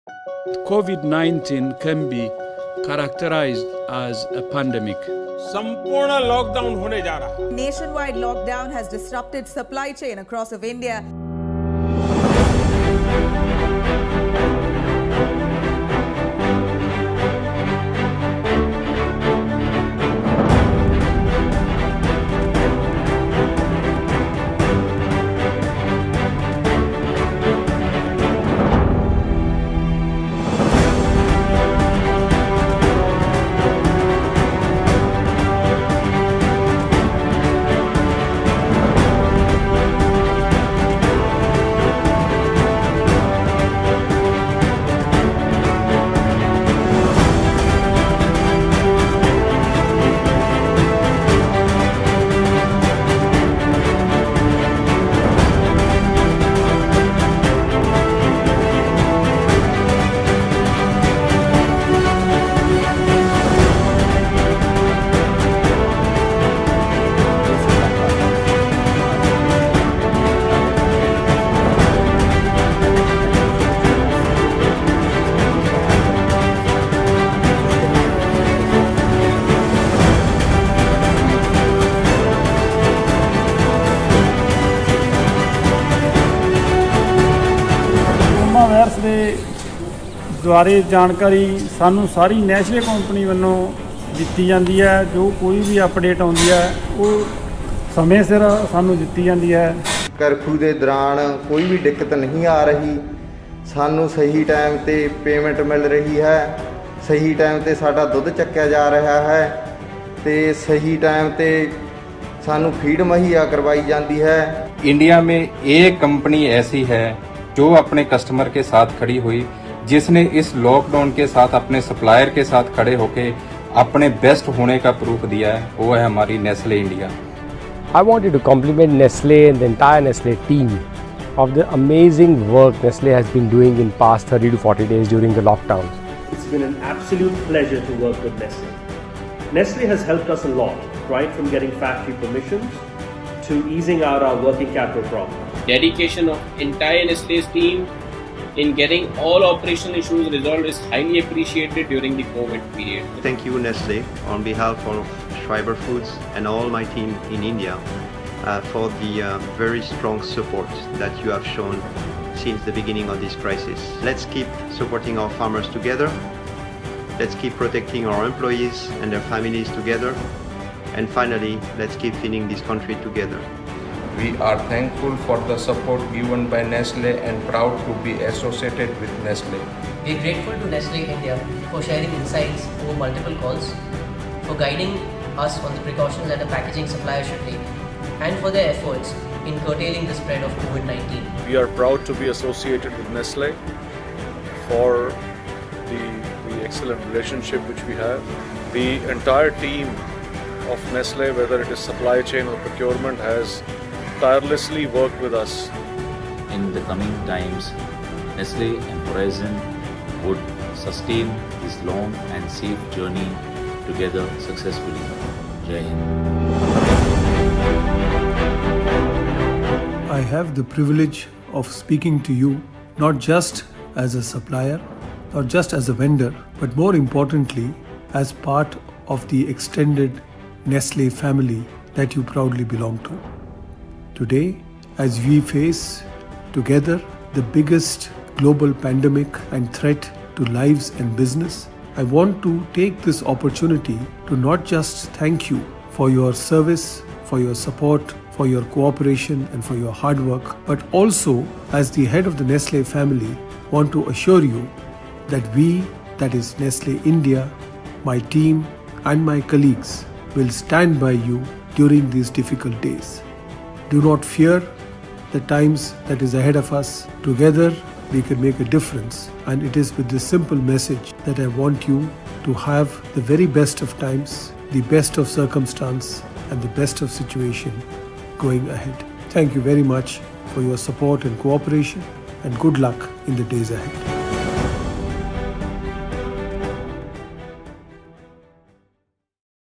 Nestle's MD Mr Suresh Narayan "Thanks" Skypack for extraordinary support during lockdowns
Skypack is proud to be in Nestle's Golden Circle - comprising of Nestle and key business partners. During Lockdowns the world was shut but Nestle and select key vendors were working hard to continue operations and diligently working to serve country by supplying key essential materials for Dairy and Food. Mr Naveen Talwar, Director Skypack expresses  gratitude in this video along with a message of Thanks by Nestle India MD Mr Suresh Narayanan for all business Partners including Skypack.